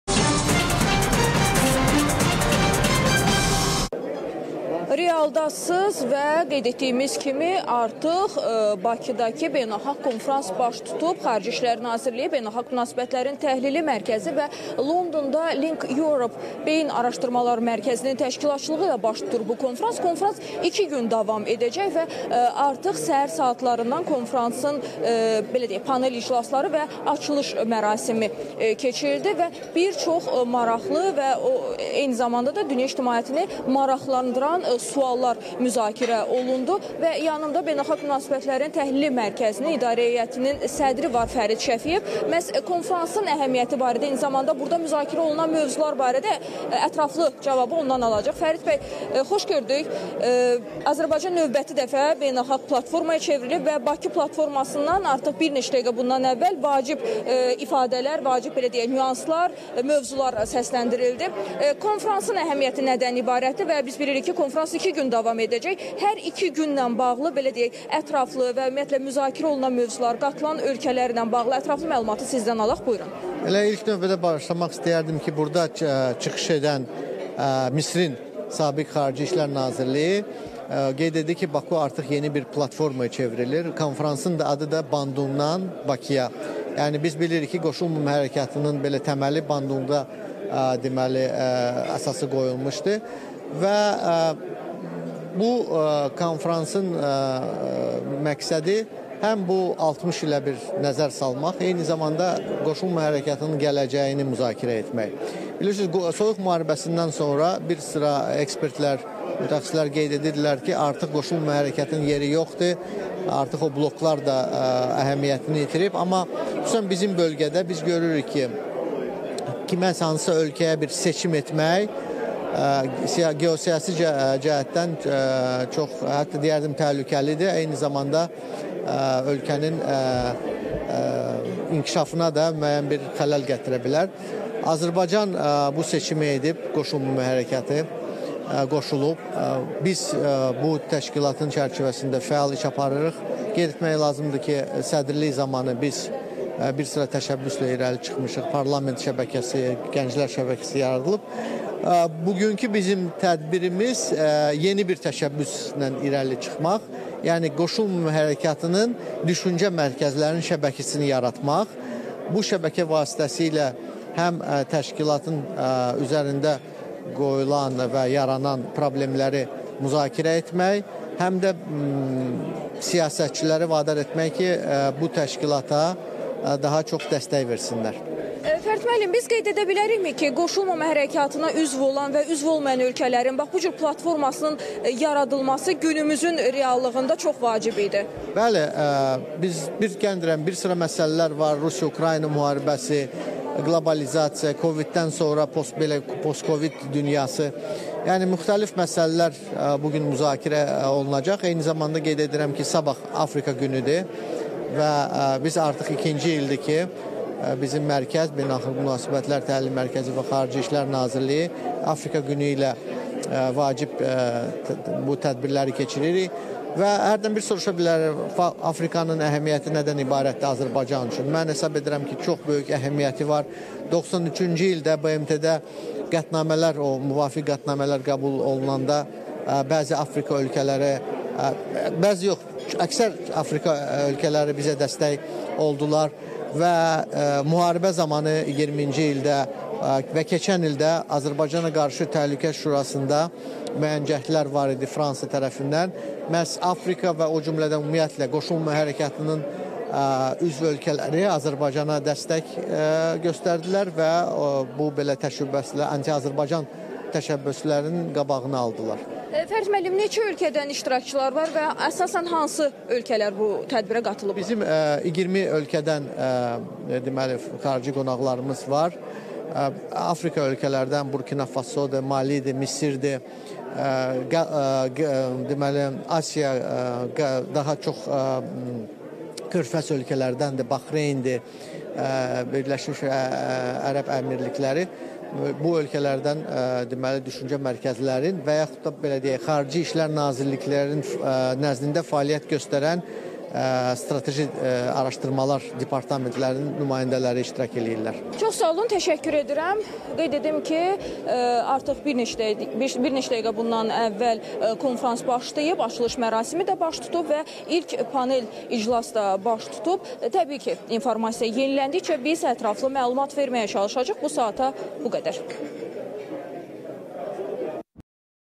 Beynəlxalq konfrans işini davam etdirir
Tədbirdə 20-dən çox ölkə iştirak edir